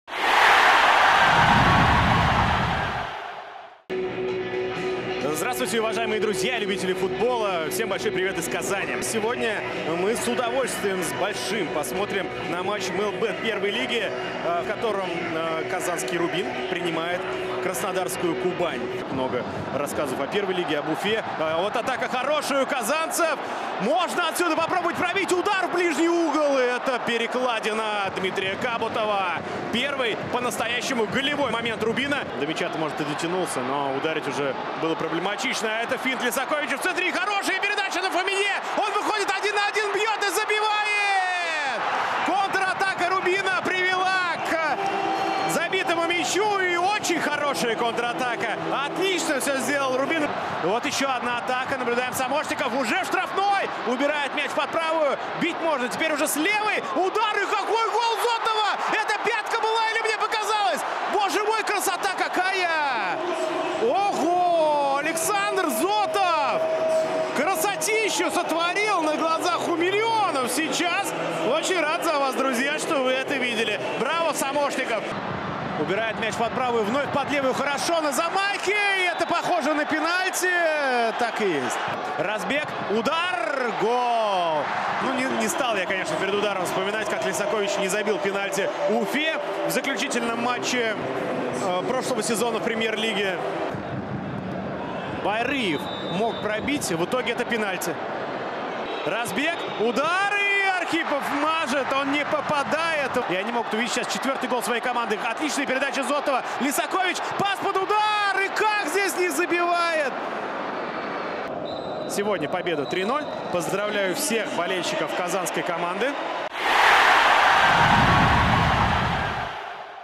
120 секунд о матче «Рубин» — «Кубань» | 5 тур МЕЛБЕТ-Первой лиги
Лучшие моменты из игры «Рубина» (Казань) и «Кубани» (Краснодар) 15 августа 2022.